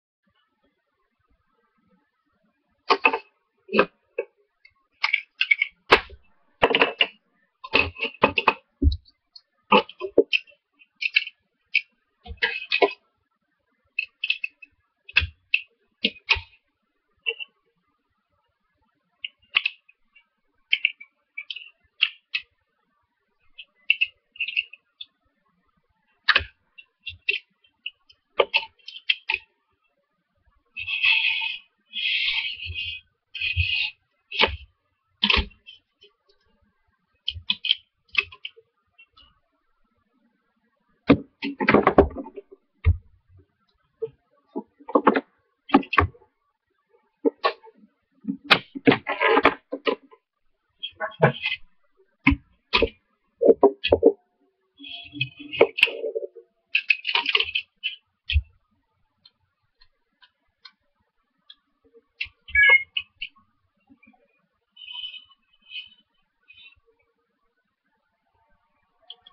Homemade Dock Stand for Ipod/Iphone with Binder clips
all you need is 2 large and 2 small clips and that is it